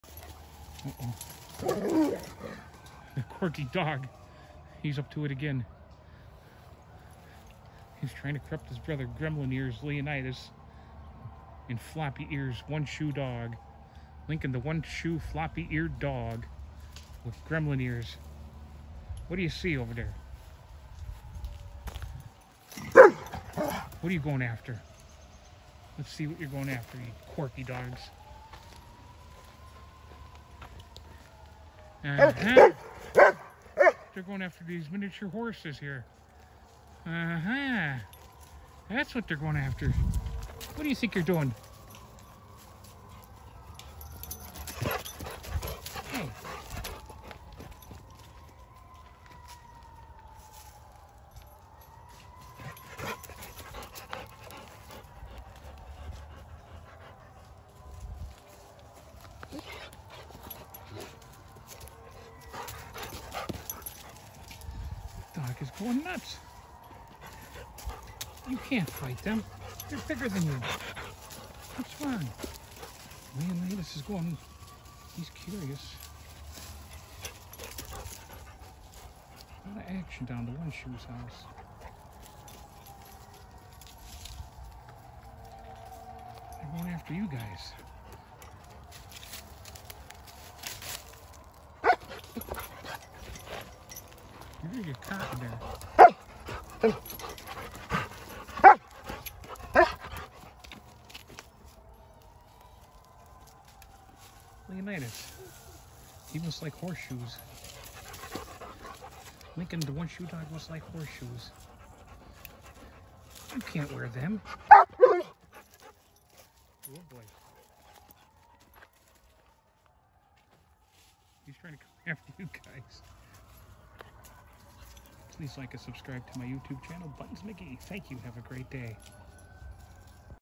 Lincoln the one shoe dog up to no good again trying to get horse shoes
Lincoln the one shoe dog up to no good again trying to get horse shoes this time and trying to corrupt gremlin ears Leonidas please like and subscribe to my YouTube channel buttons McGee